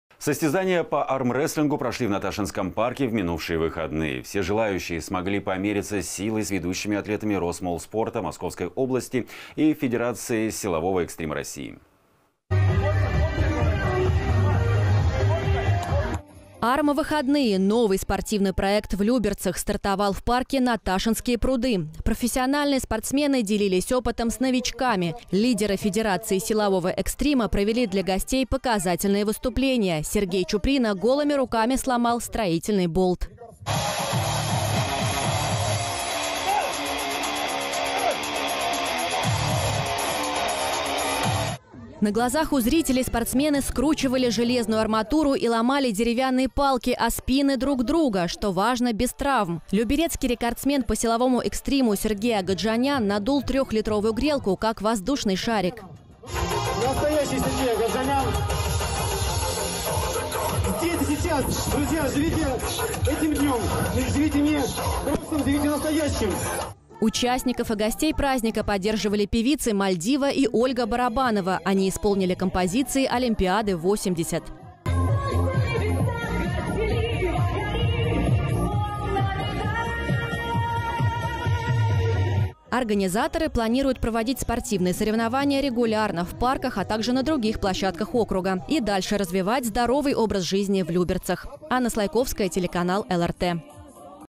Померились силой
Состязания по армреслингу прошли в Наташинском парке в минувшие выходные. Все желающие смогли помериться силой с ведущими атлетами РосМолСпорта Московской области и Федерации силового экстрима России.

«Арм-выходные» - новый спортивный проект в Люберцах. Стартовал в парке «Наташинские пруды». Профессиональные спортсмены делились опытом с новичками. Лидеры Федерации силового экстрима провели для гостей показательные выступления. Сергей Чуприна голыми руками сломал строительный болт.

На глазах у зрителей спортсмены скручивали железную арматуру и ломали деревянные палки о спины другу друга, что важно – без травм.  Люберецкий рекордсмен по силовому экстриму Сергей Агаджанян надул трехлитровую грелку как воздушный шарик.

Участников и гостей праздника поддерживали певицы Мальдива и Ольга Барабанова. Они исполнили композиции «Олимпиады-80».

Организаторы планируют проводить спортивные соревнования регулярно в парках, а также на других площадках округа. И дальше развивать здоровый образ жизни в Люберцах.